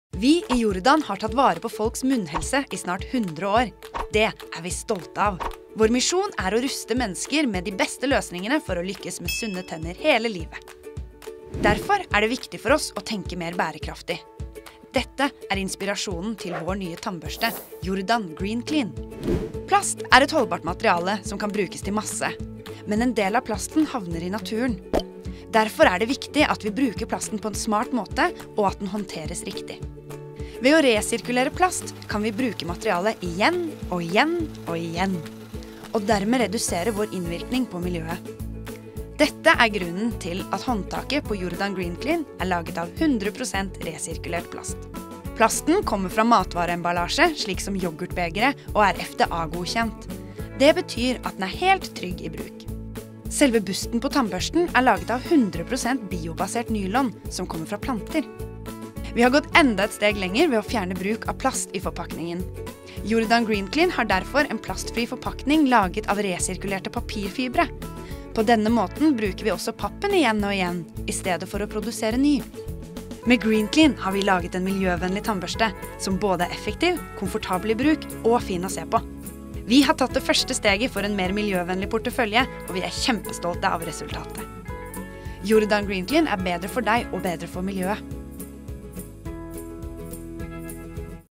Jordan Green Clean: vår miljøvennlige tannbørste
Jordan Green Clean - vår grønneste tannbørste noensinne!

Vår nye miljøvennlige tannbørste Jordan Green Clean er utformet med både deg og miljøet i tankene, slik at du kan ta vare på tennene dine med ekstra god samvittighet.

– Skandinavisk design handler like mye om estetikk som det handler om funksjonalitet. For meg er bærekraft også en stor del av det, sier Andreas Engesvik, designeren bak Jordans nye tannbørste.

BÆREKRAFT UTEN KOMPROMISS
Med sitt moderne design, ergonomiske håndtak og kvalitetsbust, kombinerer vår miljøvennlige tannbørste Jordan Green Clean funksjonalitet og bærekraftige materialer. I designprosessen har ingenting blitt overlatt til tilfeldighetene, og alle materialene er nøye valgt med miljøet i tankene.

Faktisk er hele 90 prosent av det totale produktet (emballasje inkludert) laget av resirkulert materiale, noe som minimerer ressursbruk og gir materialer nytt liv. For eksempel er tannbørstehåndtaket laget av resirkulert plast som stammer fra matbeholdere, og emballasjen er laget av resirkulerte papirfibre fra papirbaserte produkter.

De resterende 10 prosentene består hovedsakelig av børstehårene, som er 100 prosent biobaserte, og emballasje-etiketten som er laget av FSC-sertifisert papir fra ansvarlig forvaltede skoger.